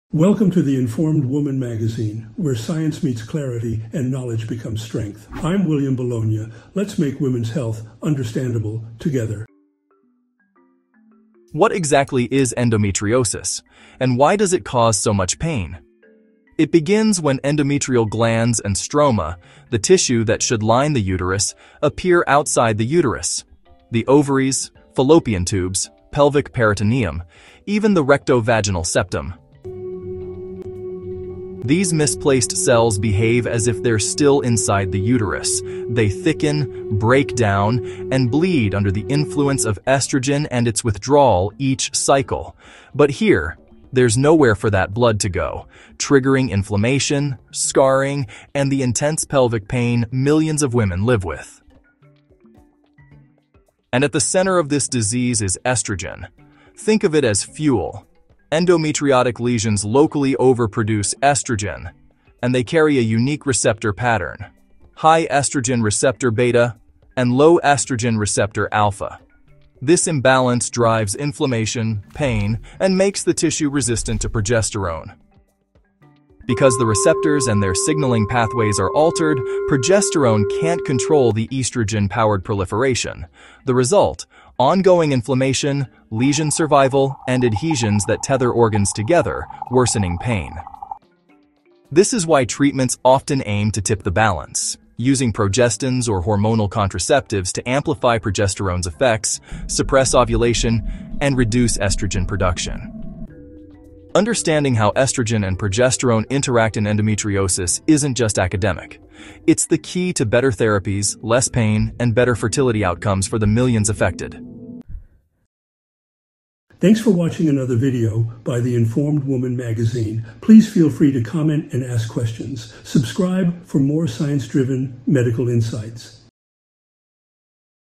Endometriosis Inside Out: Estrogen, Progesterone & Pain
Endometriosis occurs when endometrial glands and stroma grow outside the uterus, where they respond abnormally to estrogen and progesterone. Estrogen acts as fuel, driving inflammation, lesion growth, and persistence through local estrogen production and a unique receptor imbalance (high ER-beta, low ER-alpha). Meanwhile, progesterone, normally responsible for regulating endometrial stability, loses its effectiveness. This “progesterone resistance” prevents progesterone from countering estrogen’s effects, allowing continued inflammation, pain, and adhesion formation. Hormonal treatments aim to suppress estrogen and strengthen progesterone pathways, but resistance makes management complex. Understanding this interplay is key to better therapies and improved fertility outcomes.